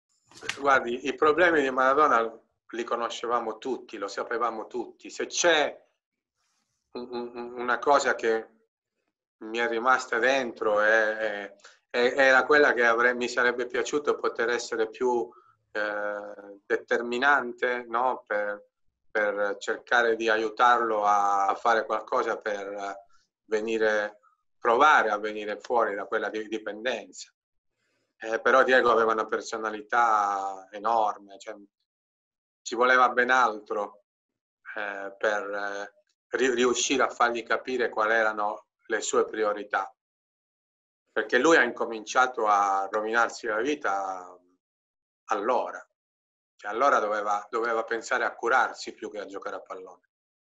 'Diego started ruining his life at Napoli' - Maradona's teammate | Naples | Italy | Serie A | 马拉多纳
Massimo Mauro, who was Diego Armando Maradona's team-mate at Napoli between 1989 and 1991, remembered the late Argentinian legend on Thursday, praising his on-pitch heroics and ruing the addiction that 'ruined' his life.
Massimo Mauro moved from Juventus to Napoli only to have the chance to play alongside Maradona.
He won a Serie A title and an Italian Supercoppa with the Argentinian champion.
When Maradona joined Napoli in 1984, the club had won virtually nothing and was far removed both geographically and socio-economically from the country's soccer capitals of Milan and Turin.
If there's one place outside Argentina that will likely match - or possibly even exceed - the outpouring of mourning for Diego Maradona, it's in Naples.